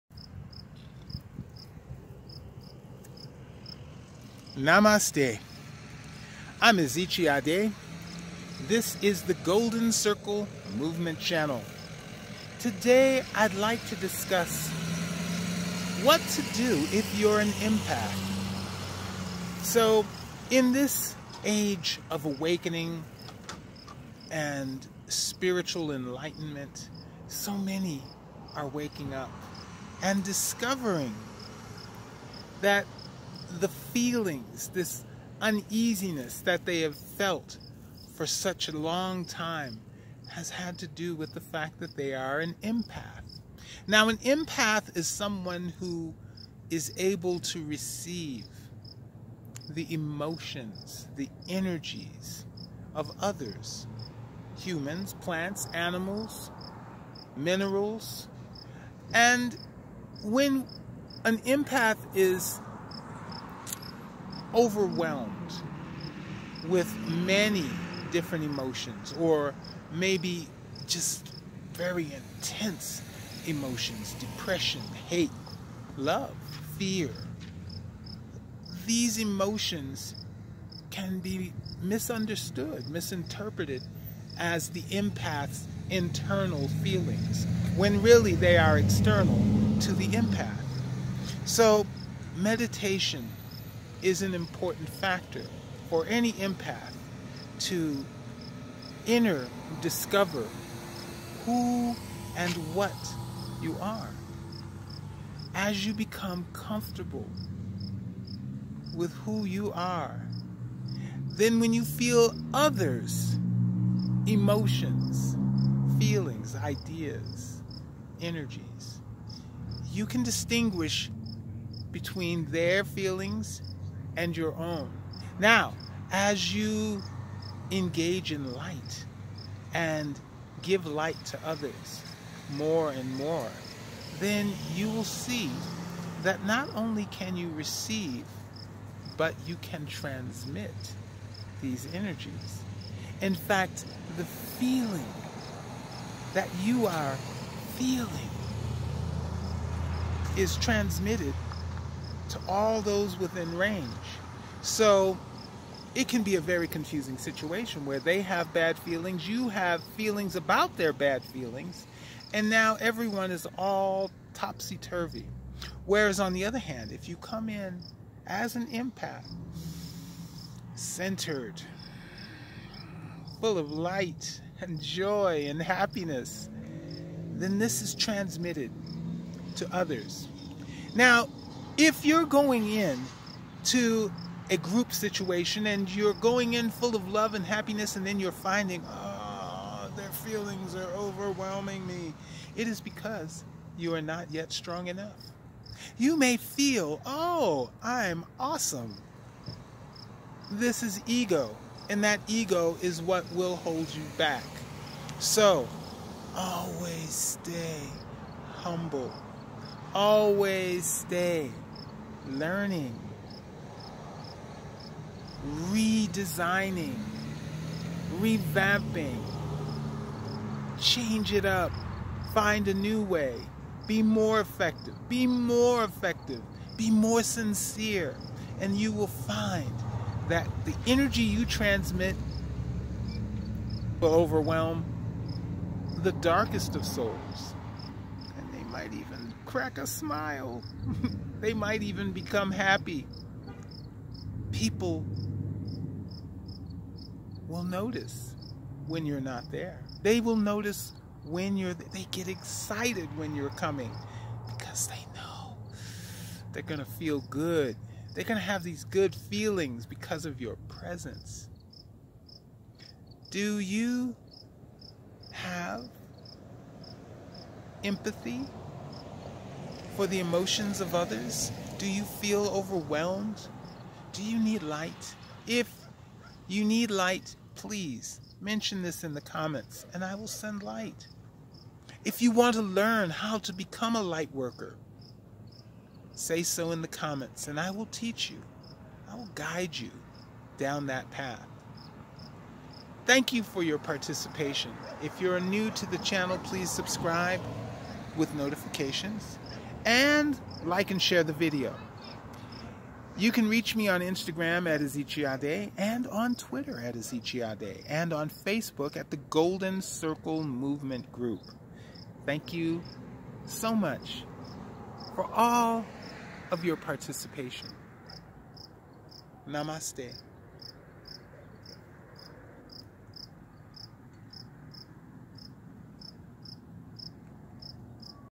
What to do if you're an empath
Namaste. When I find myself overwhelmed by the emotions of the crowd, I go within and create joy.  Realizing I can transmit as well as receive, the Light I am able to radiate changes the emotional States of those around me creating a bubble of Paradise.

Join the discussion on Facebook. What subjects should I discuss?

Your donations help with production costs and contribute to making The Golden Circle Movement even better.  Thank You. Namaste